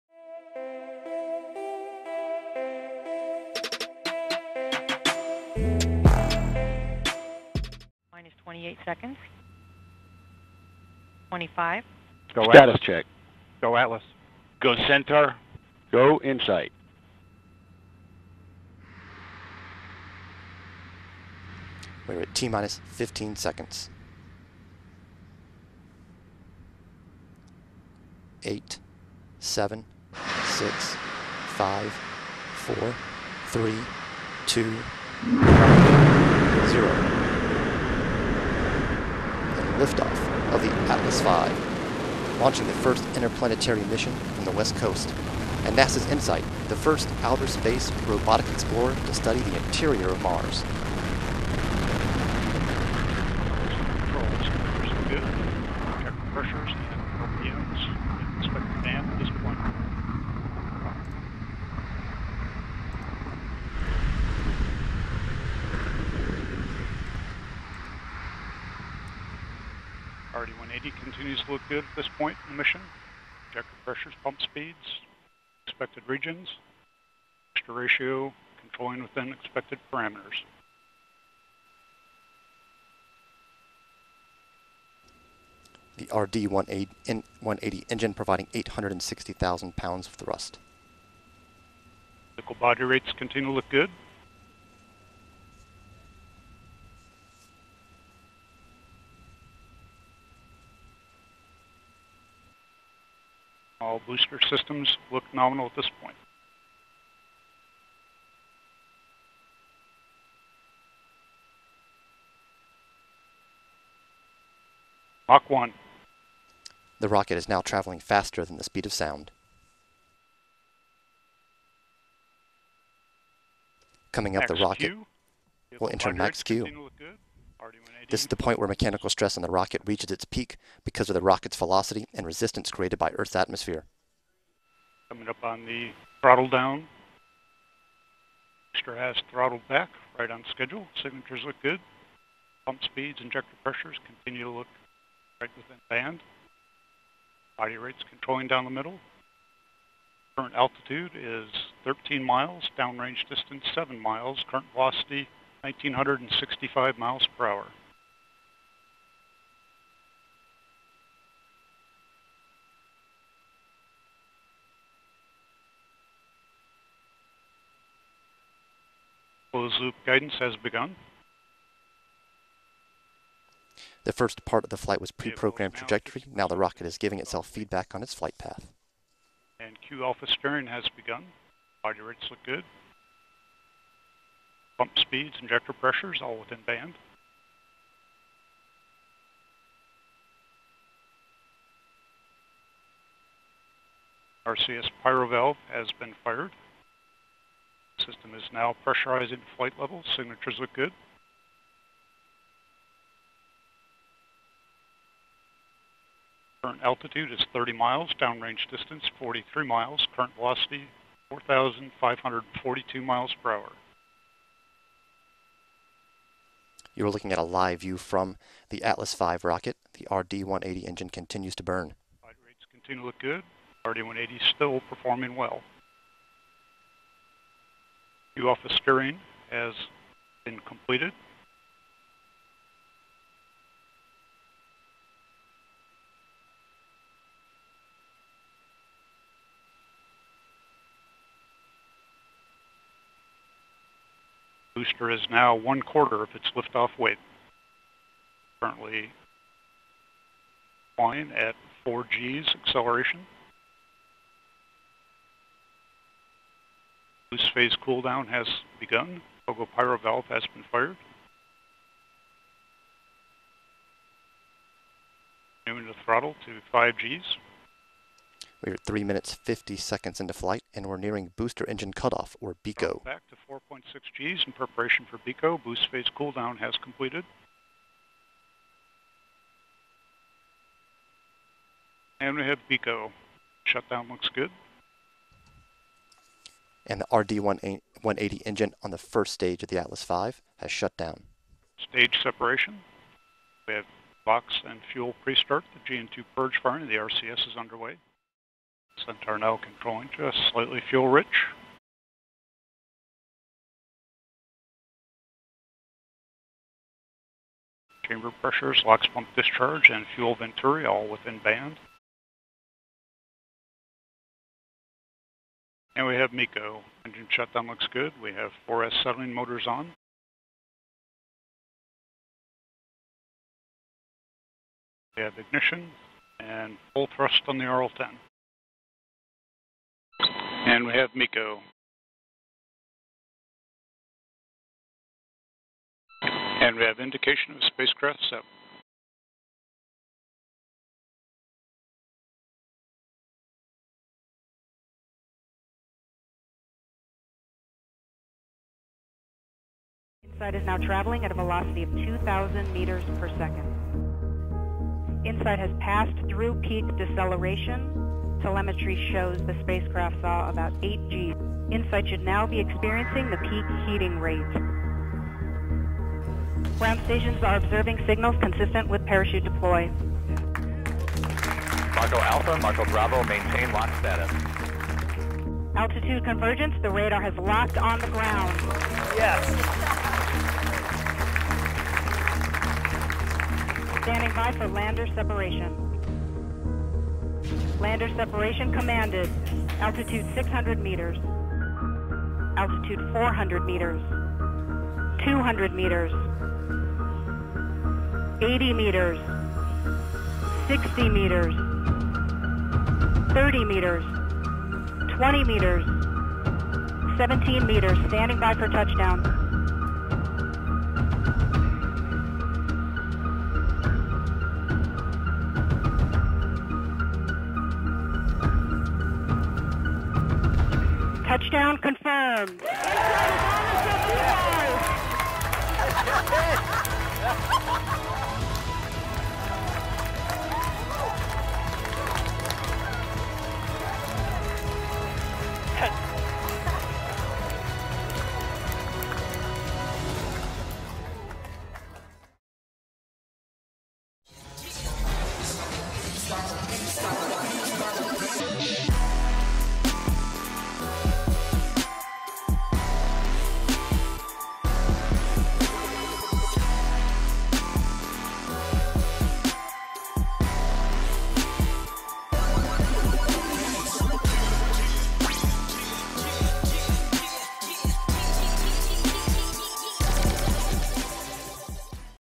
NASA InSight - From Launch to Landing on Mars
NASA's InSight lander arrived on the Martian surface to learn what lies beneath the red planet. Insight launched atop ULA's Atlas V rocket from Vandenberg Air Force Base in California on May 5, 2018. The lander touched down on Mars on November 26, after performing a perfect and critical entry and descent through the Martian atmosphere to gently touch down near Mars' equator on the western side of a flat, smooth expanse of lava called Elysium Planitia at 11:52 a.m. PST (2:52 p.m. EST).

Video edit: Go To Space
Footage Courtesy: NASA / JPL / ULA

- Where clothing 👕 meets the Cosmic fabric 💫. Never forget: Space is our Future 🧑‍🚀 Go To Space 🚀